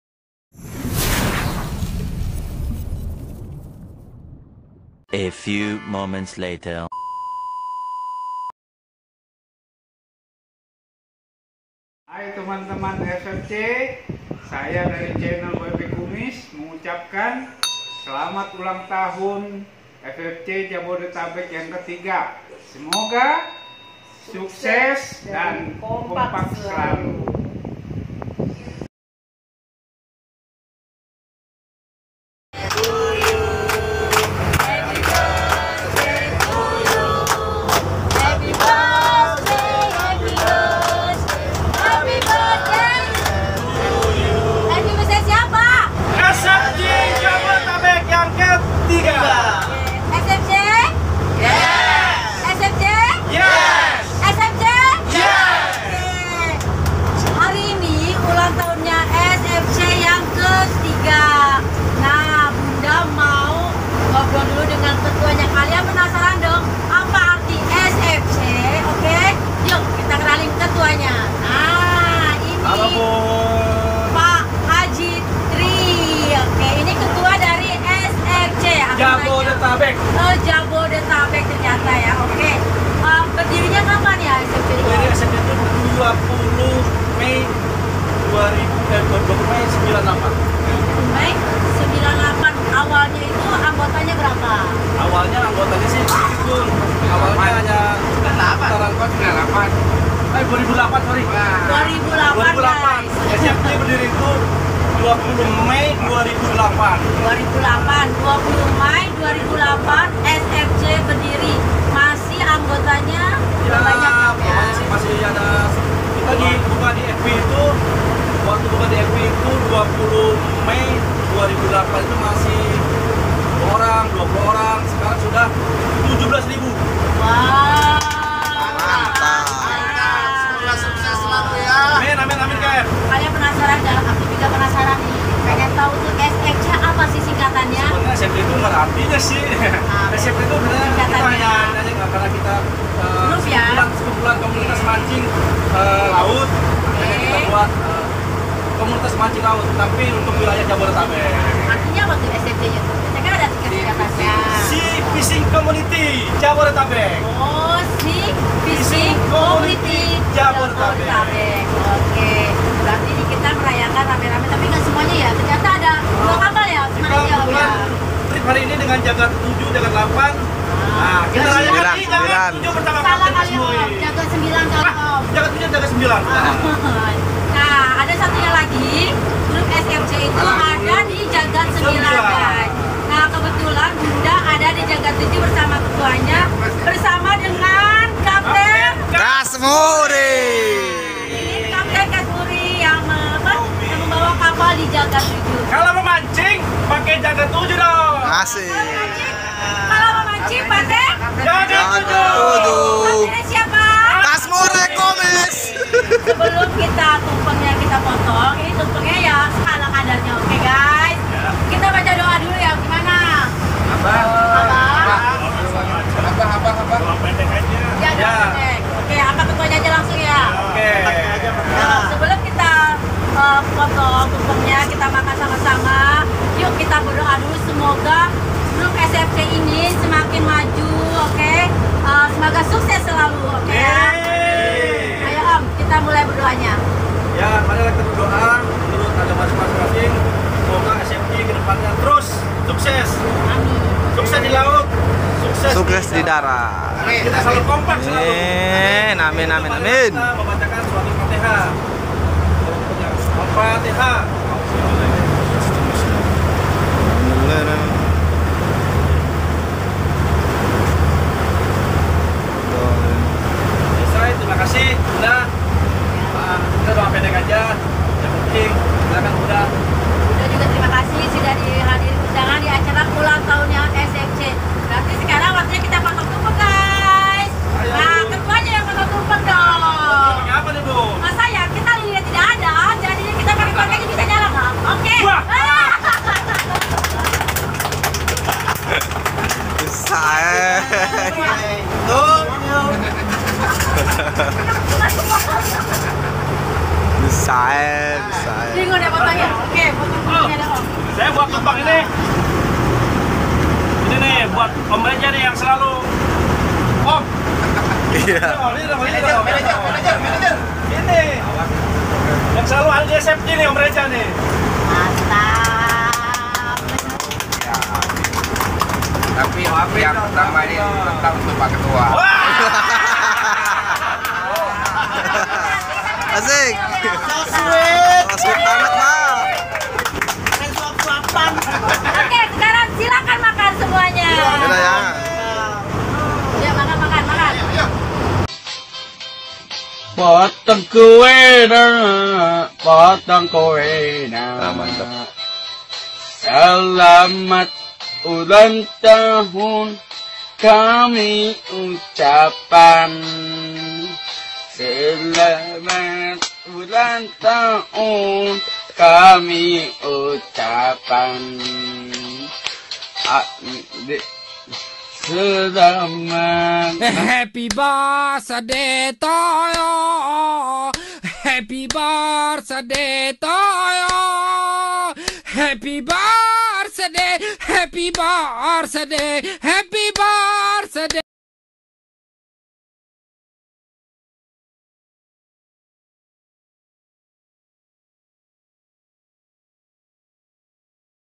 MANCING BERSAMA ULANG TAHUN SFC KE 3
Mancing bersama sambil merayakan ulang tahun SFC ( Sea Fishing Comunity ) ke 3 di tengah laut dengan menyewa 2 kapal mancing

terpaksa Mancing bersama ini karna ada keperluan saya gak bisa ikut padahal sudah DP 😢

yang penting selamat ULTAH KE 3 TETAP KOMPAK TETAP SEMANGAT